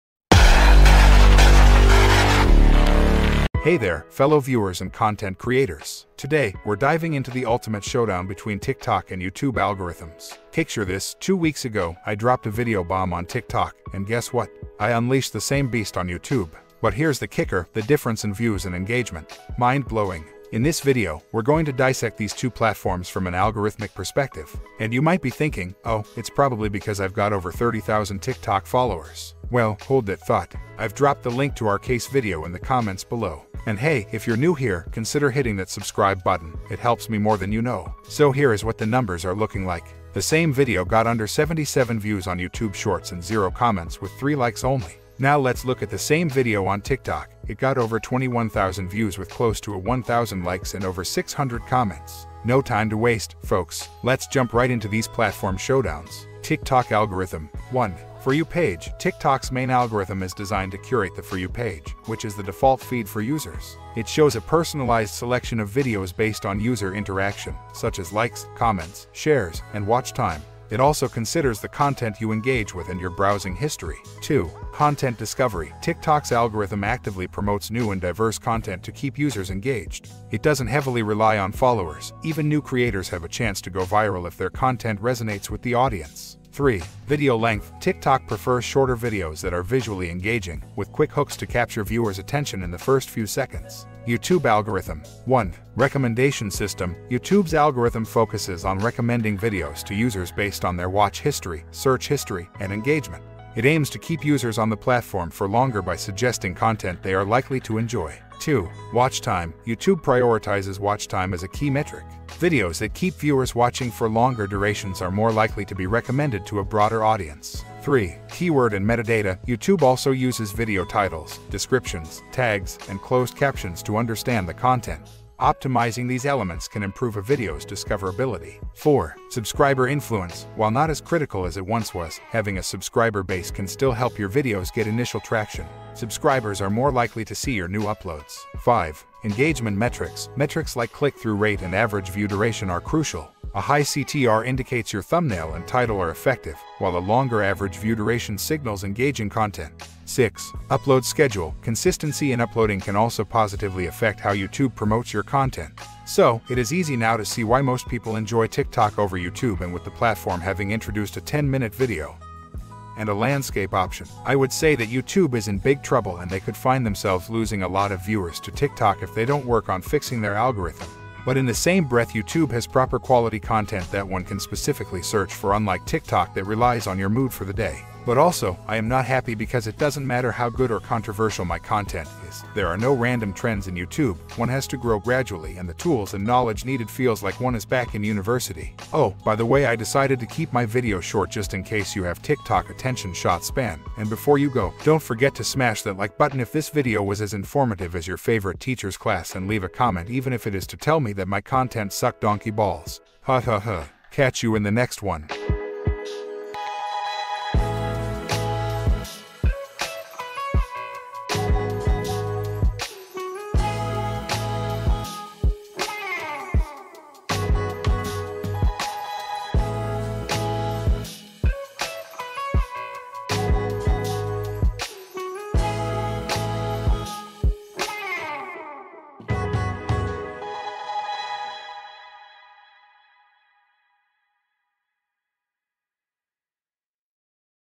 Same video uplodaded on Tiktok and YouTube but shoking analytics.
In this video, we're diving deep into the mysterious worlds of TikTok and YouTube algorithms. Ever wondered why your content performs differently on these platforms?

Join me as we demystify the algorithms, comparing the tricks and treats each one has up its sleeve. From engagement to visibility, we'll uncover it all!

Don't miss out on the exclusive insights and real-world data we've collected. Watch till the end for a better understanding of how to boost your content's reach.

Find all the relevant links and resources mentioned in the video below. Let's chat!